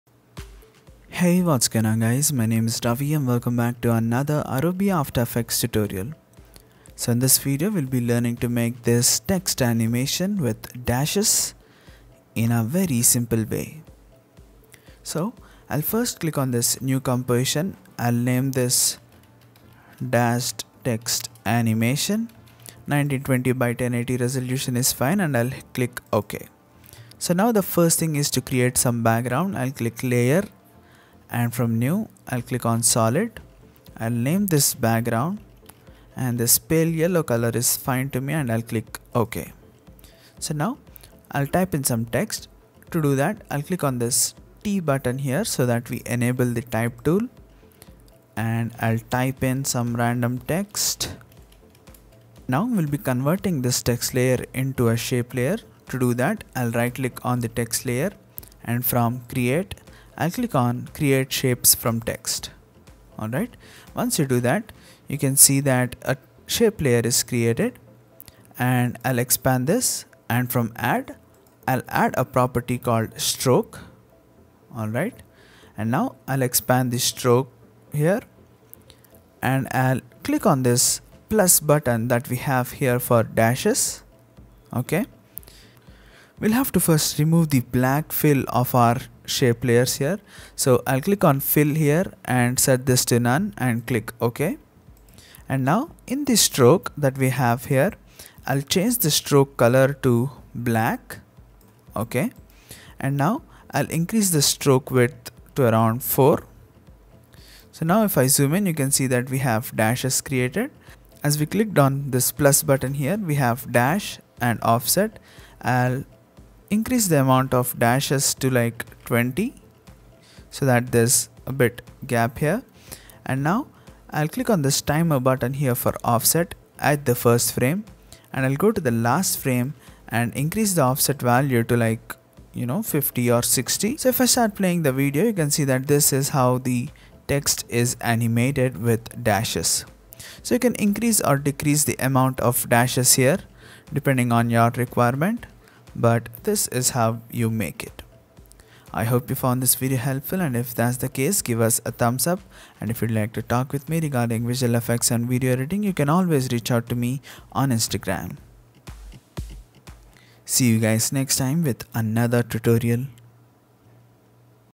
How to make Dashed Text Animation | Adobe After Effects Tutorial
Learn How to Make Dashed Text Animation using Adobe After Effects. In this adobe after effects tutorial, we will be learning to make dashed text animation in a very simple and easy way. We will be converting our text layer to a shape layer and will add stroke property to generate this effect.

➔ Stay Connect With iraFX Studio